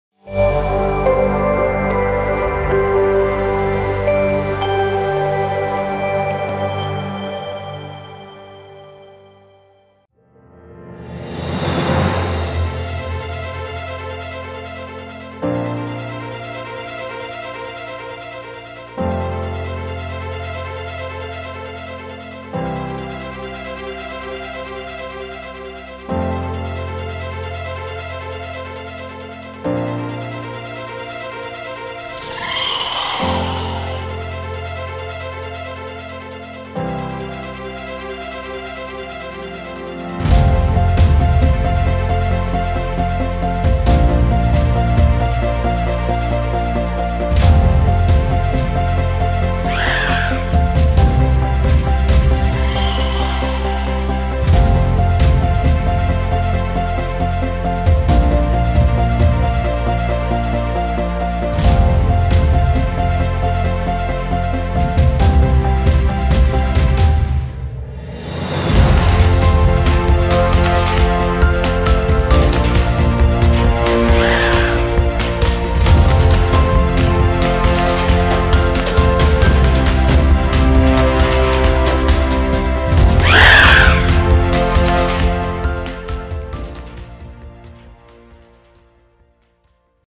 Sky Fountains - Pterodactylus Art Gallery
Dynamic composition Sky Fountains with flying pterodactyls in the art gallery.